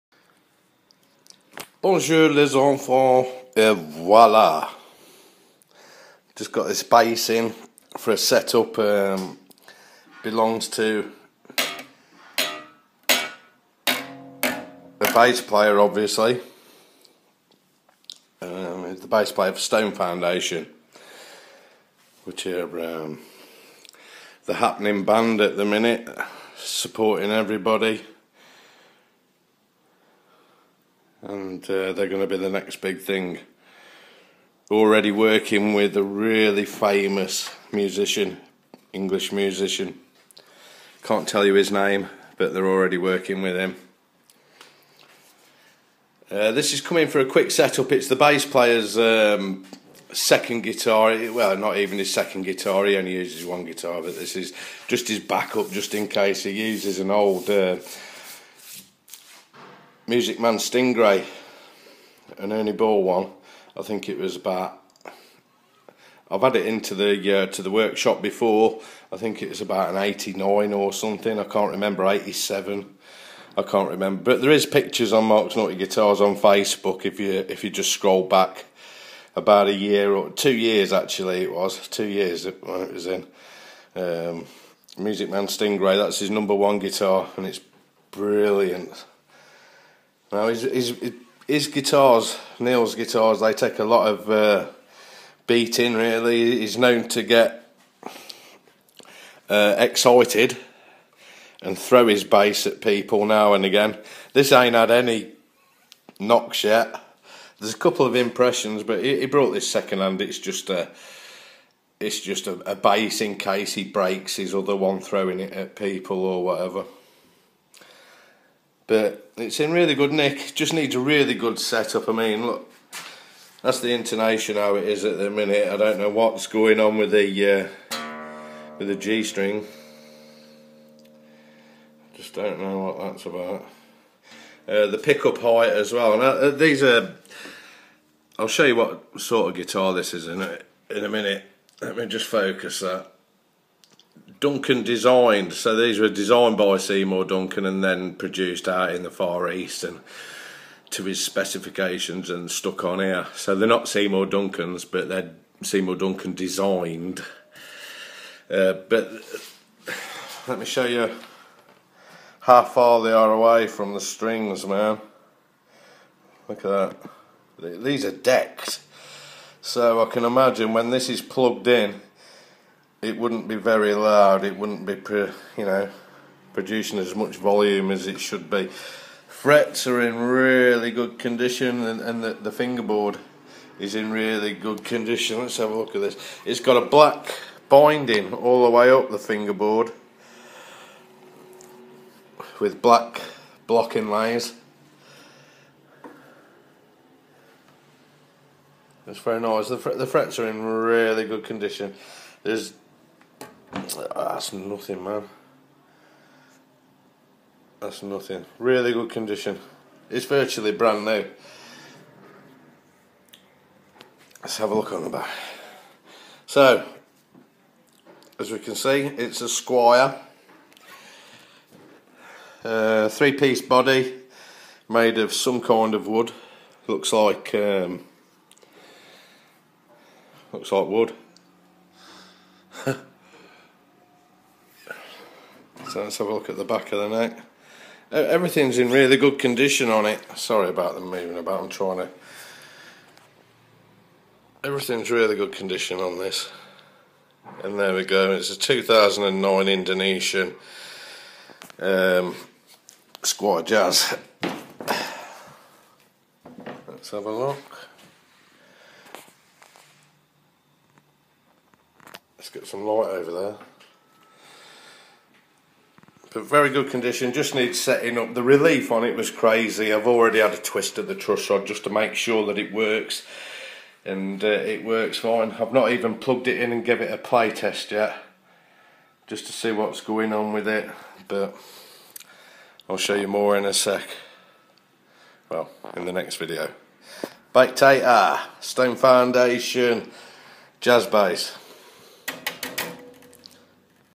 Squier Jazz bass 2009
Stone Foundation guitar in for a set up.